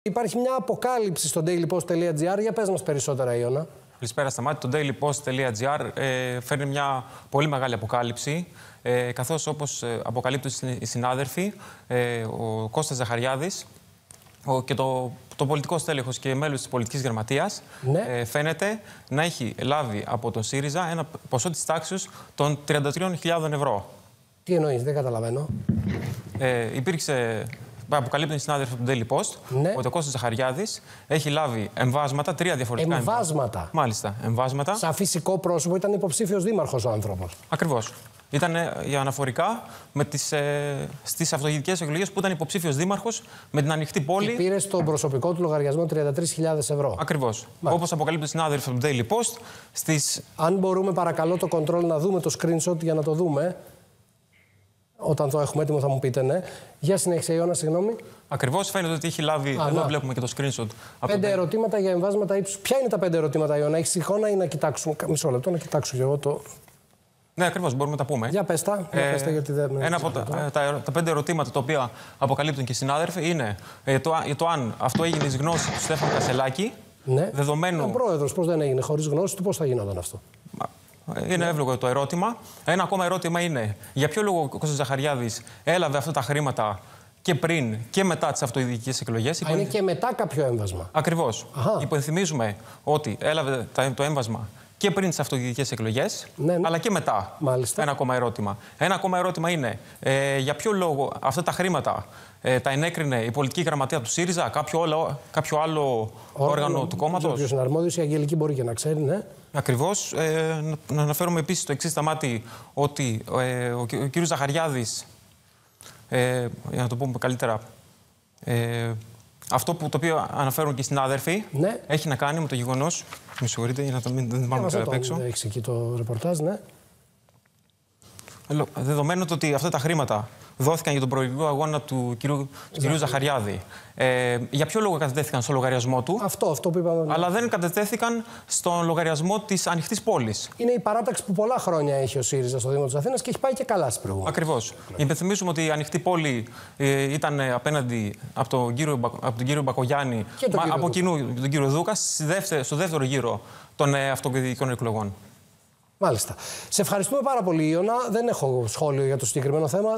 ΣΥΡΙΖΑ: Ερωτήματα για τα εμβάσματα ύψους 33.000 ευρώ στον Κώστα Ζαχαριάδη | One Channel
Η αποκάλυψη του dailypost.gr για το έμβασμα 33.00 ευρώ στο μέλος της Πολιτικής Γραμματείας του ΣΥΡΙΖΑ Κώστα Ζαχαριάδη. 20/9/2024